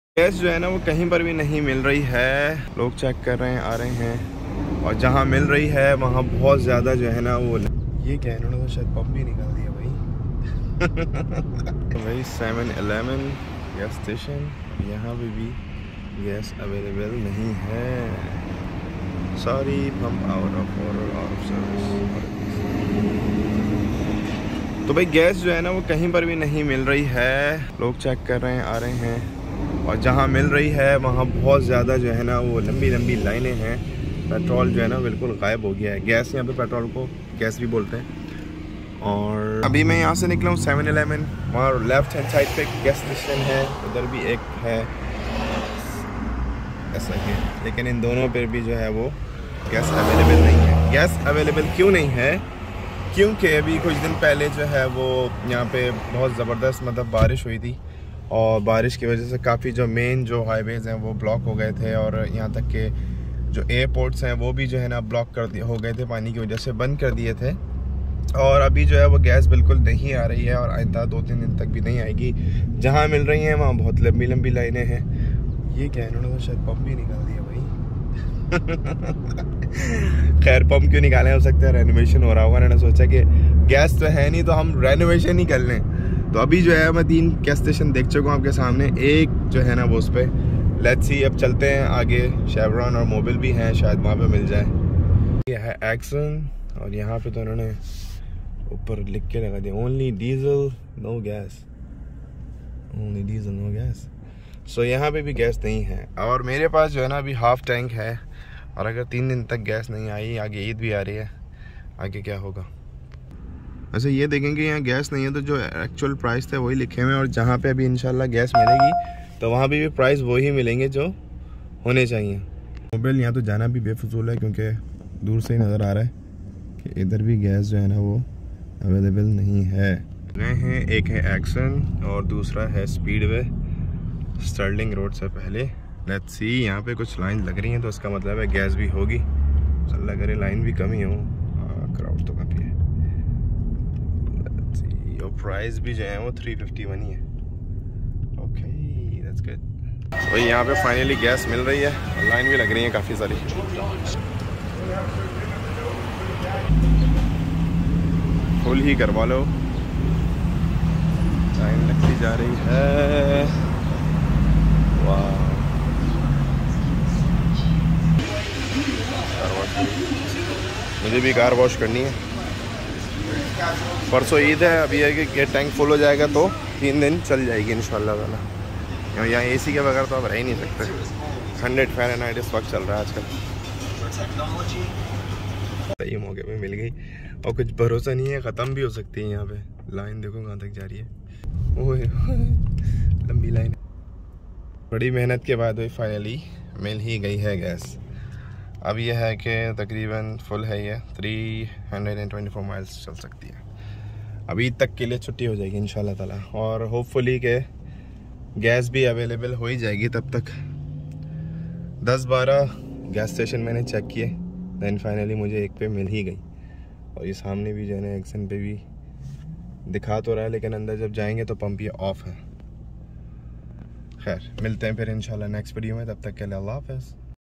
Florida Shortage Gas ⛽️ 😒
From Palm Beach to Key West, gasoline shortages have started to pop up as a result of last week's torrential rainfall that flooded parts of South Florida for several days including Fort Lauderdale and Port Everglades, which provides 40% of the gasoline for the whole state